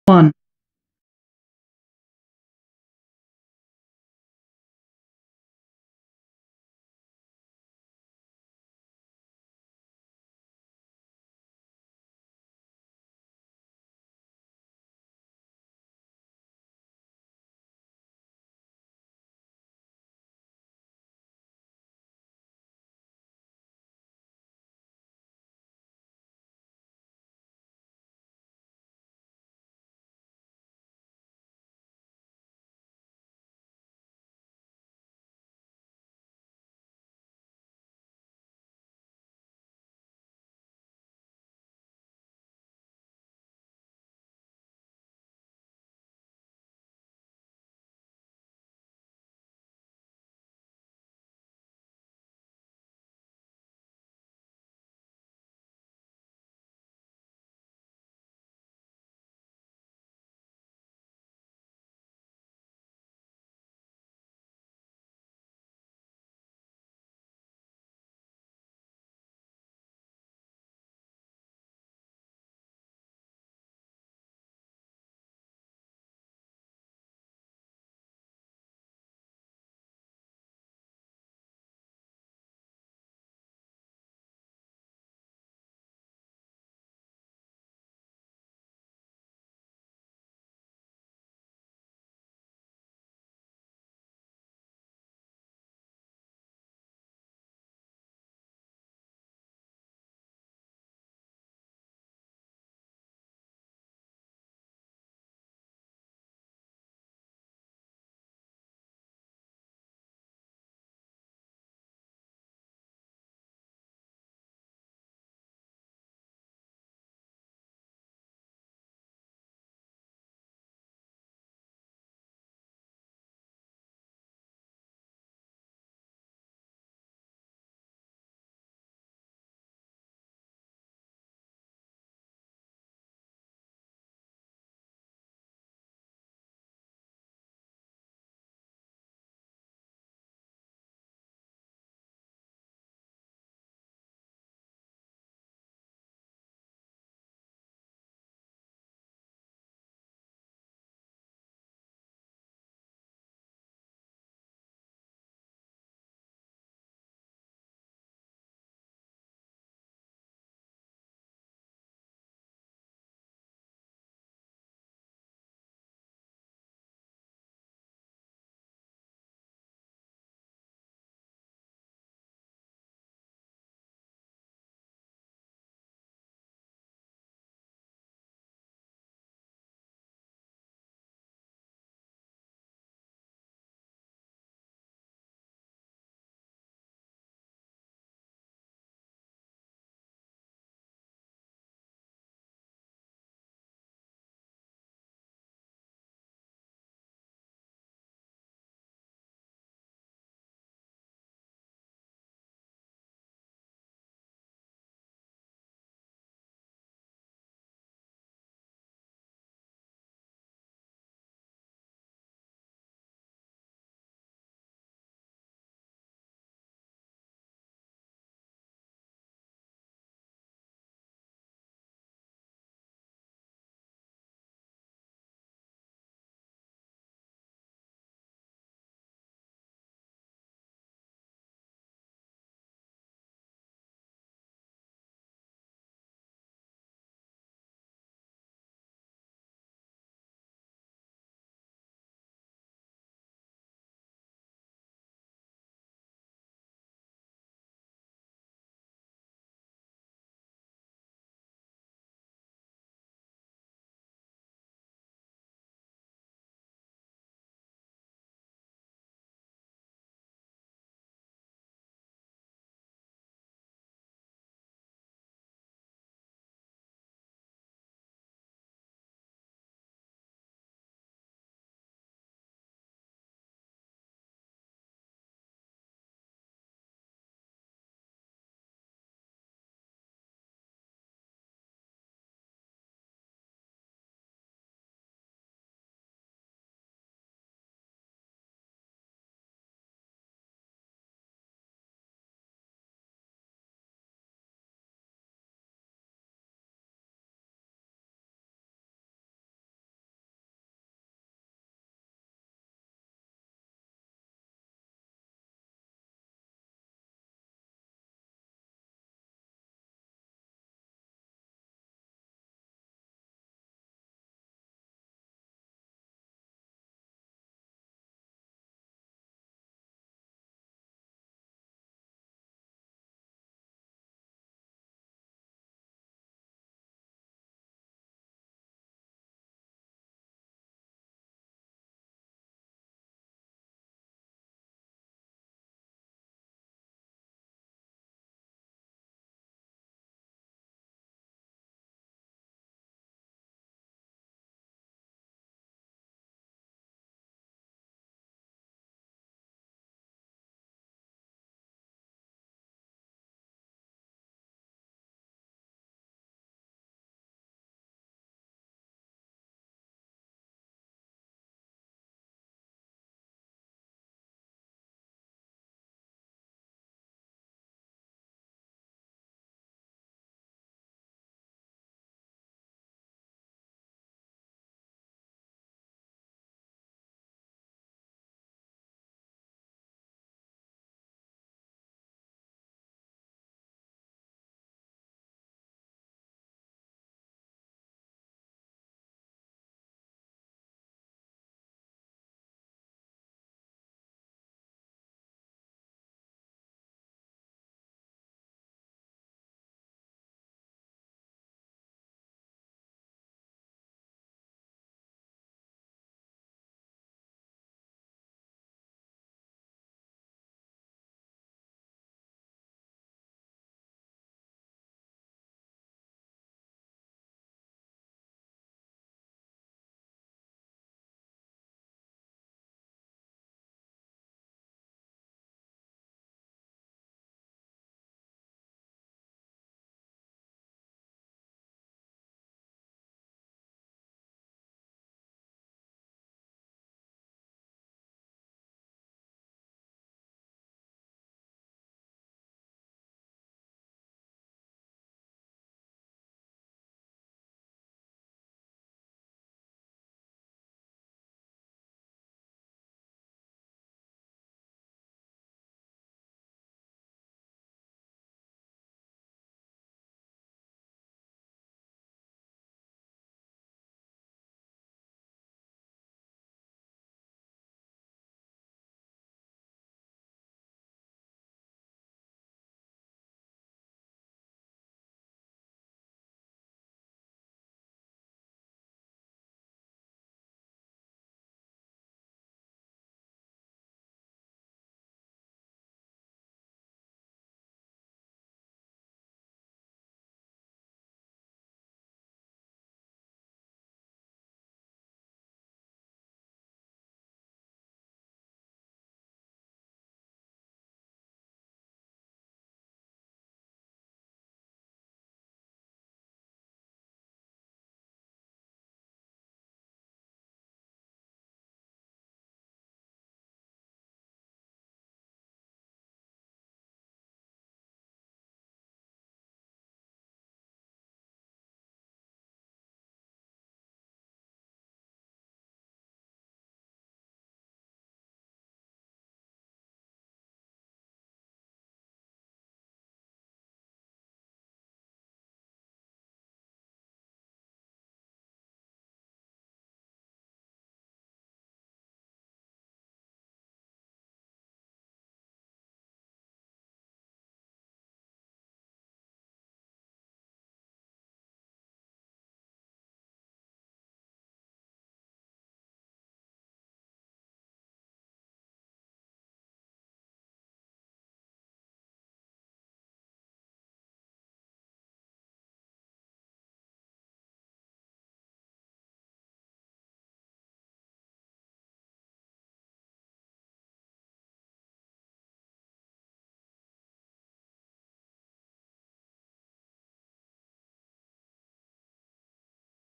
Canadian Fiscal Rules across global operations Jan 6 2018 9 44 AM
How to assess the business climate for Canadian Region. The taxation rules, as well as the merits of investing in Quebec.